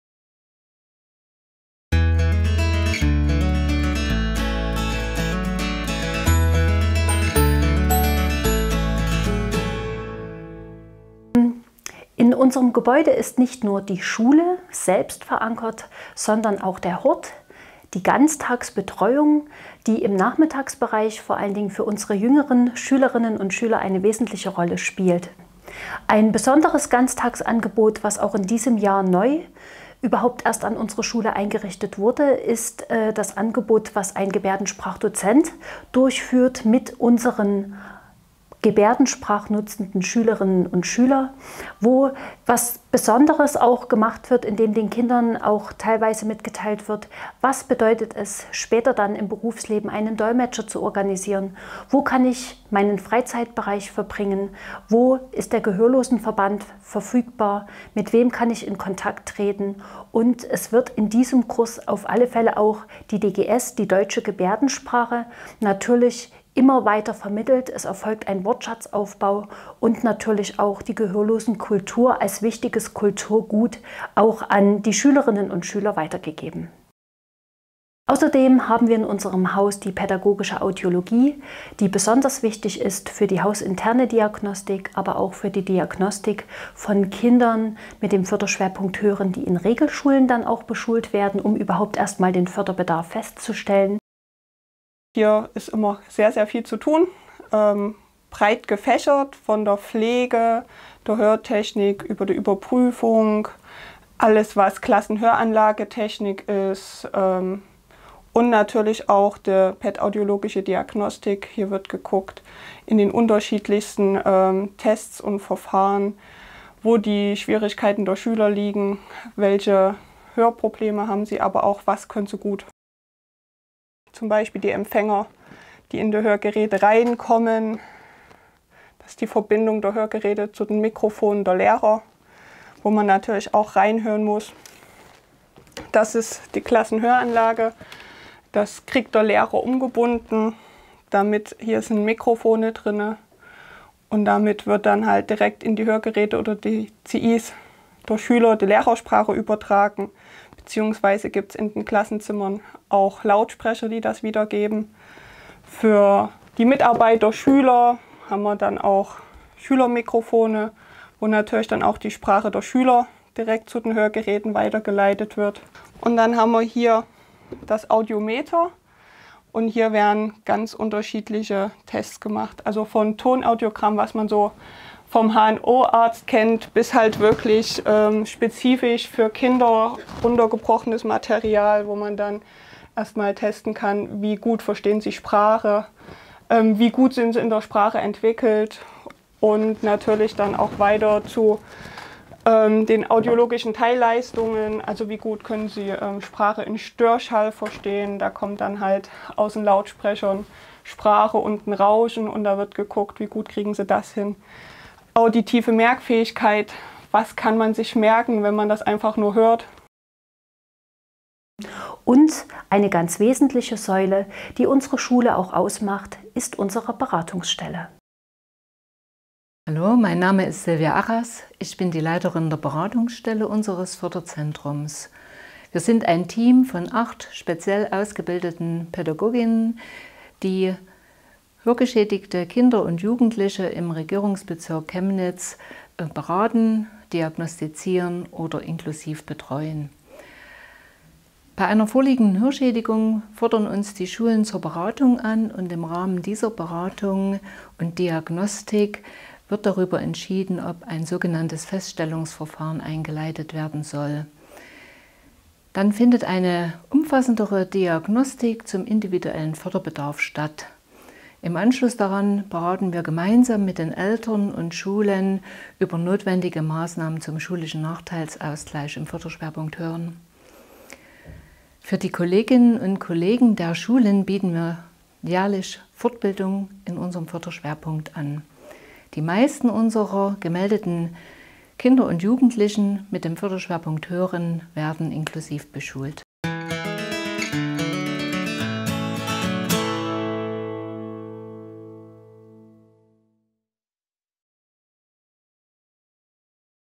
Förderzentrum »Georg Götz« Chemnitz II
LEHRER WERDEN IN CHEMNITZ? – Teil 2 | Im zweiten Teil schauen wir uns die besonderen Angebote des Förderzentrums »Georg Götz« Chemnitz genauer an.

Der Gebärdensprachdozent

»Ein weiteres, besonderes Ganztagsangebot bietet seit Kurzem ein Gebärdensprachdozent für alle gebärdensprachnutzenden Schülerinnen und Schüler an«, freut sich Schulleiterin Anja Wünsche. »In dem Gebärdensprachkurs wird die DGS, die Deutsche Gebärdensprache, vermittelt. Dazu gehört zum Beispiel der Wortschatzaufbau. Hier wird aber auch die Gehörlosenkultur an die Schülerinnen und Schüler weitergegeben. Die Mädchen und Jungen lernen auch, was es bedeutet, im späteren Leben einen Dolmetscher zu organisieren und wie sie Kontakt zum Gehörlosenverband aufnehmen können.«

Die Audiologie

Das Förderzentrum verfügt über eine pädagogische Audiologie. Sie ist besonders für die hausinterne Diagnostik wichtig. »Sie ist aber auch für die Diagnostik von Kindern, die später in Regelschulen beschult werden, von Bedeutung, um den Förderbedarf festzustellen.«

Die Beratungsstelle

»Wir sind ein Team aus acht speziell ausgebildeten Pädagoginnen«, erzählt die Leiterin der Beratungsstelle, Sylvia Arras. Sie und ihr Team sind für die Diagnostik und inklusive Betreuung hörgeschädigter Schülerinnen und Schüler verantwortlich. Das heißt: Sie besuchen die Kinder und Jugendlichen vor Ort in ihrer Heimatschule im Regierungsbezirk Chemnitz und beraten dort Lehrerinnen und Lehrer sowie Eltern zu allen Fragen, die sich aus der Hörschädigung der Kinder ergeben. Dieser mobile Dienst kann sowohl von Schulen als auch von Eltern angefordert werden. Nach Feststellung des Förderbedarfs werden die Kinder und Jugendlichen kontinuierlich betreut. Außerdem bietet das Team der Beratungsstelle Fortbildungen für Lehrerinnen und Lehrer an Regelschulen an.

❓ Wie werde ich Lehrerin bzw. Lehrer in Chemnitz? Mehr dazu – auch zum Förderzentrum »Georg Götz« – gibt es in unserem Blog